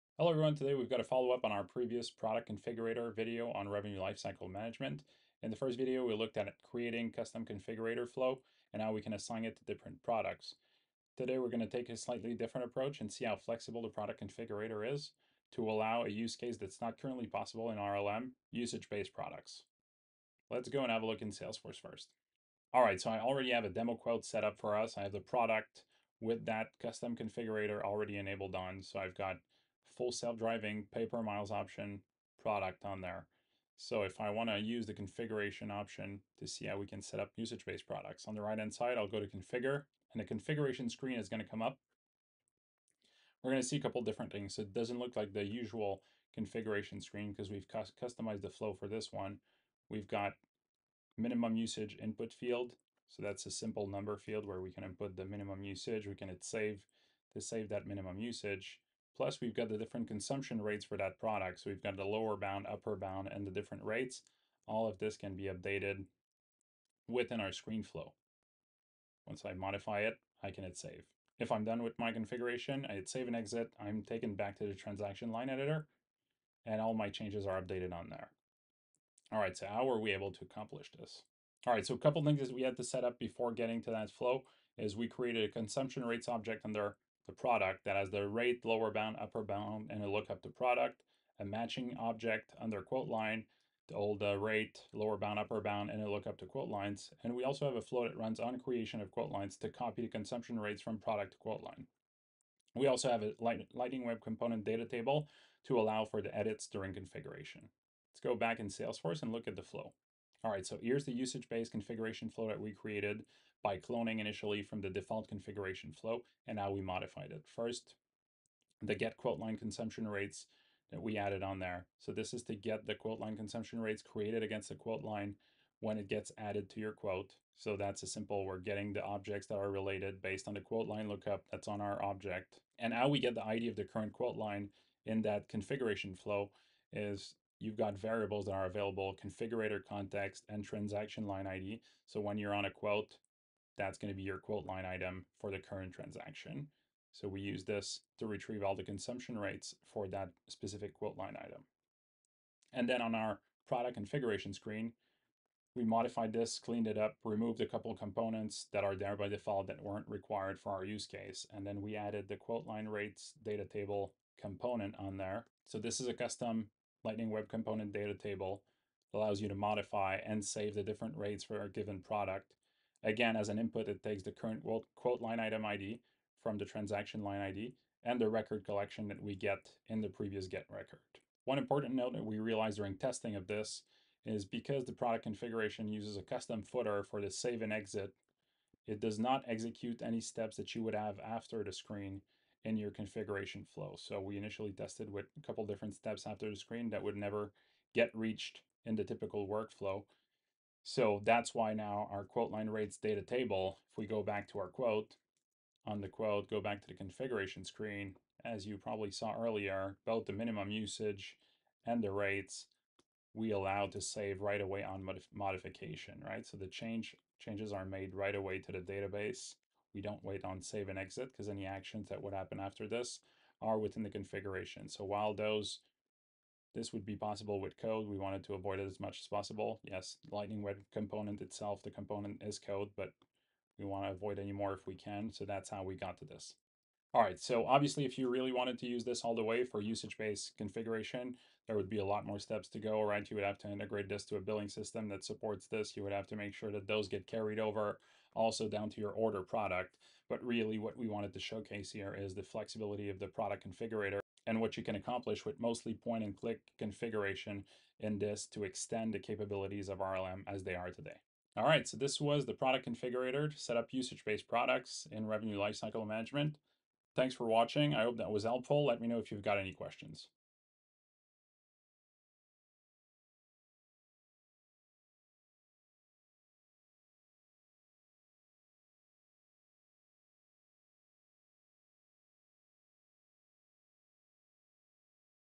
Usage Based Products with Salesforce Revenue Lifecycle Management
In this video, we dive into how to configure and manage usage-based products in Salesforce Revenue Lifecycle Management (RLM), even though they aren't supported out of the box as of Summer '24.

Leveraging the flexibility of Salesforce's Product Configurator, we'll show you step-by-step how to set up and manage products based on usage, such as our example "Self-Driving Pay-per-Mile" product.

From customizing flow screens to using Lightning Web Components for real-time data updates, we’ll cover the essential techniques to make usage-based billing a reality in your RLM setup.